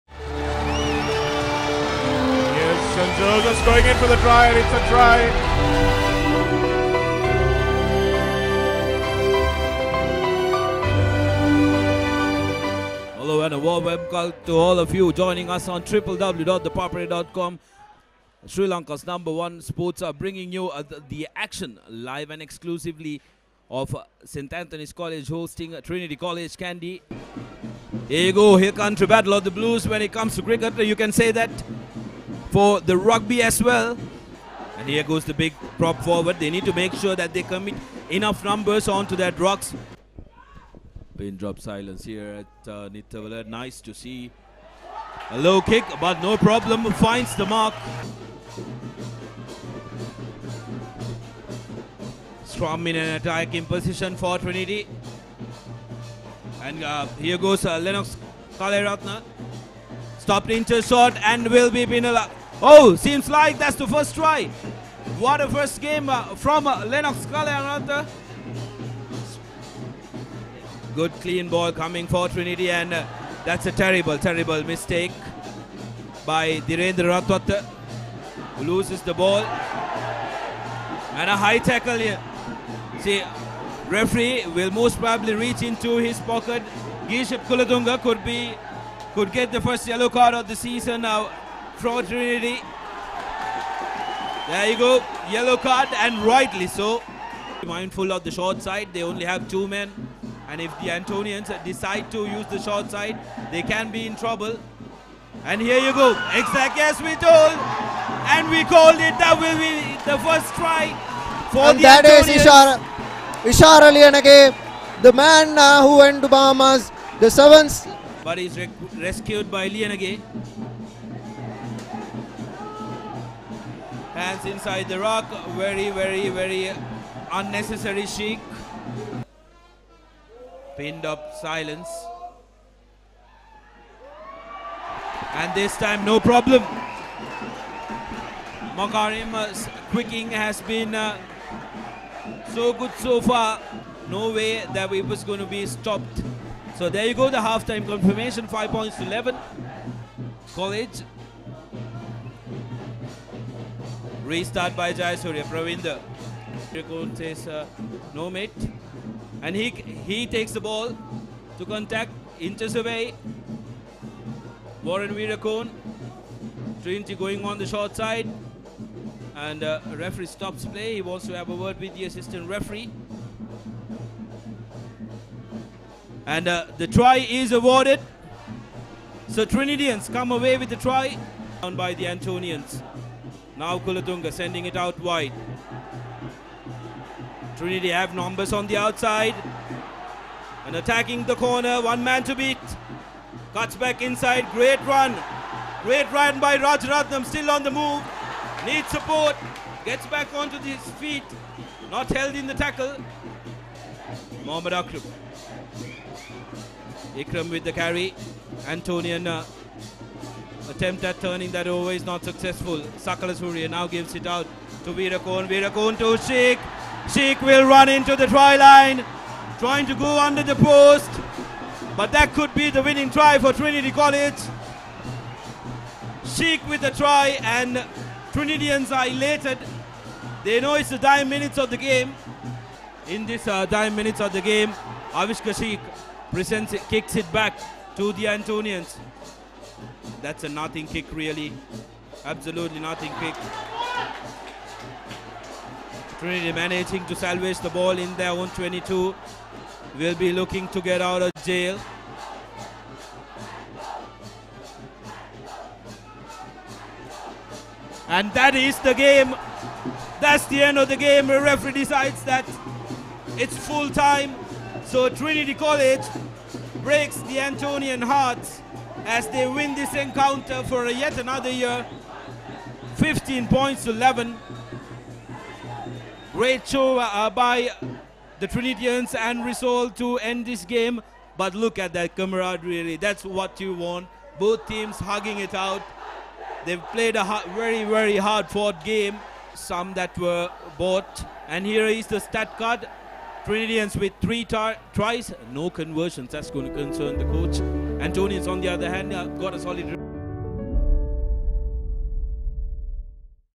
Match Highlights – St. Anthony’s College v Trinity College | Schools Rugby 2018 #5
Match Highlights from the St. Anthony’s College v Trinity College encounter in the Schools Rugby League played on the 12th of March 2018 at Nittawela.